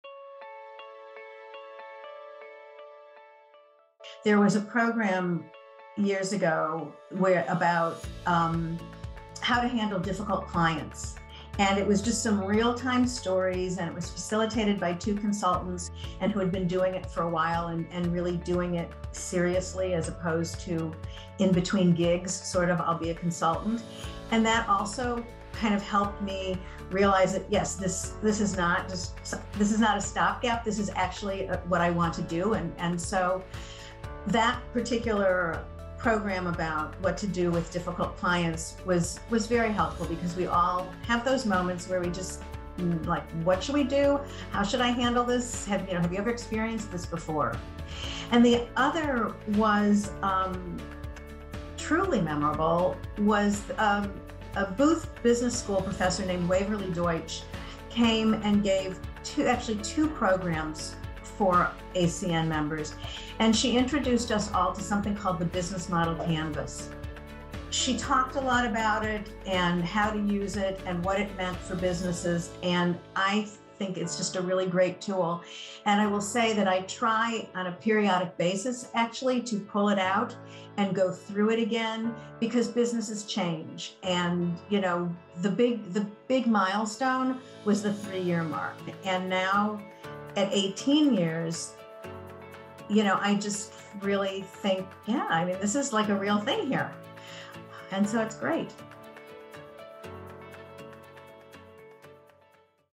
ACN's Value: Amy Wishnick's Take Away from Programming
Amy Wishnick has been a member of ACN since the start of her career in nonprofit consulting. She tells us about the value she has received from the programming offered, providing examples of topics that help her to this day with her work!